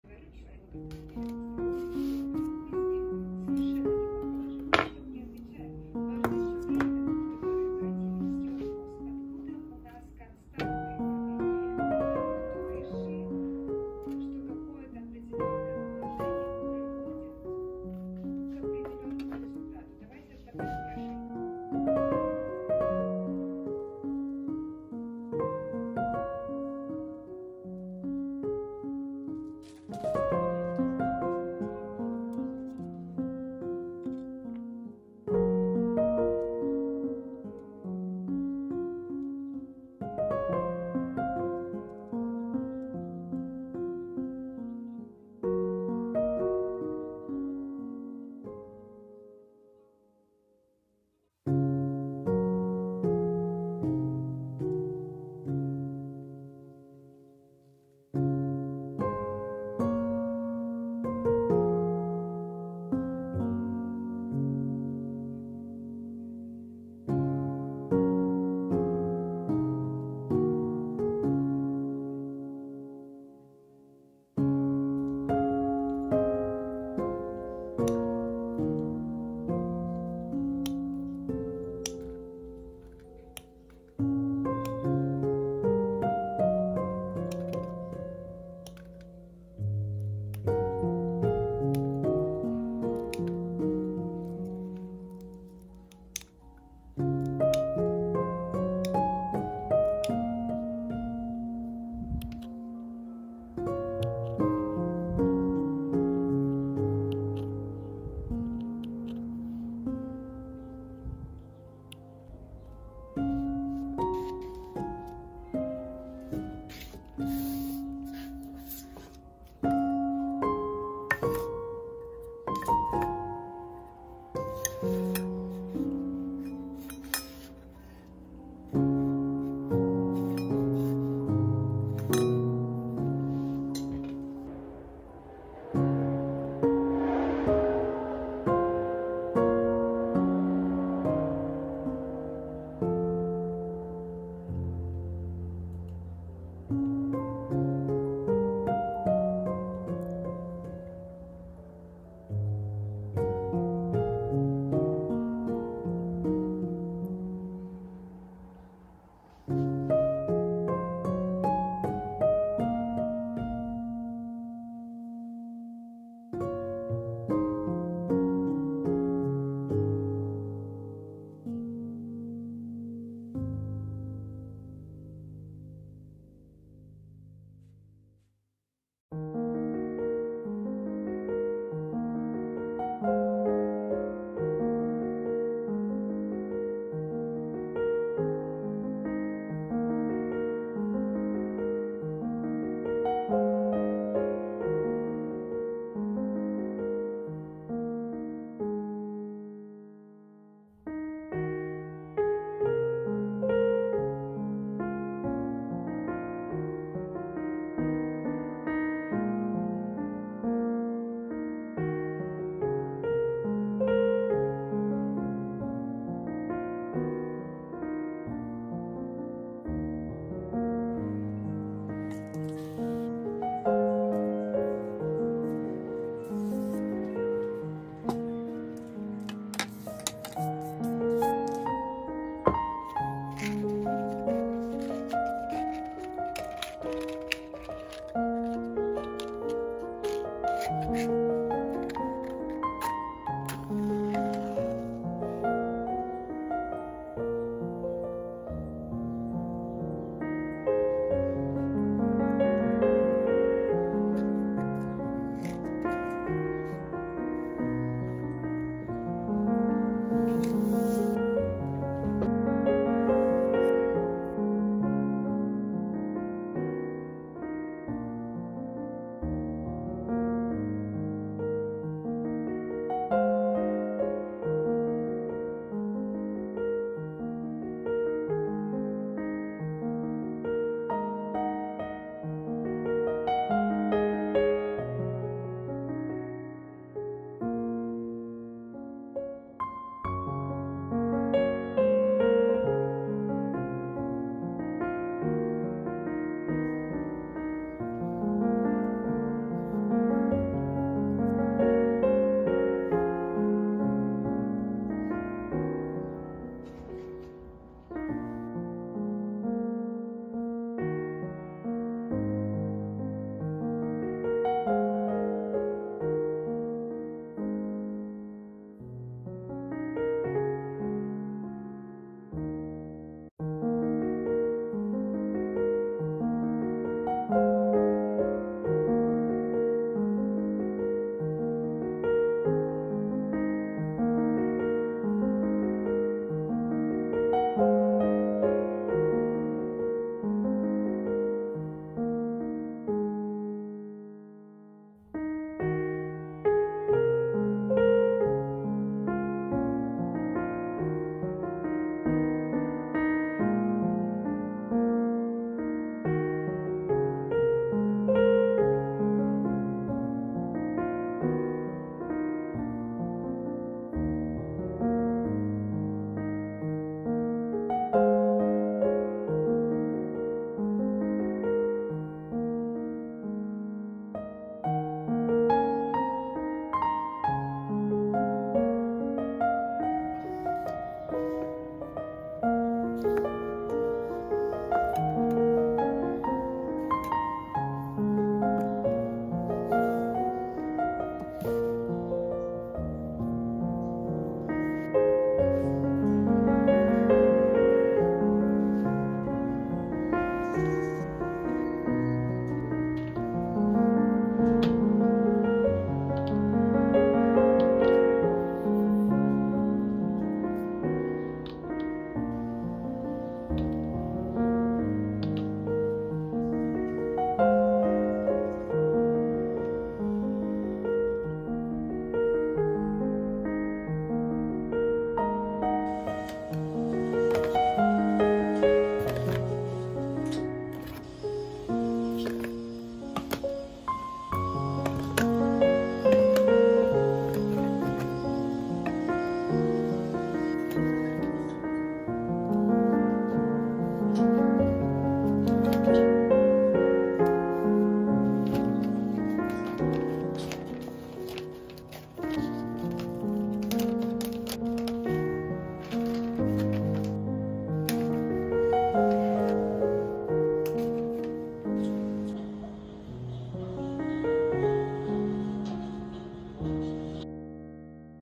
Making and packing 150 soy candles | big Etsy order
In this video I wanted to show some working moments. I was making 150 pcs soy-bees wax blend candles in dandelion chamomile scent. It took me 3 days to make and pack this huge order. Wasn't really into explaining something, the weather is a bit gloomy for the past few days and this summer reminds me of some days form the past. Somewhat summertime sadness I would say. I think you can perfectly fill my mood through this video, so get yourself a cup of tea or coffee and listening to this relaxing music with me!

Etsy shop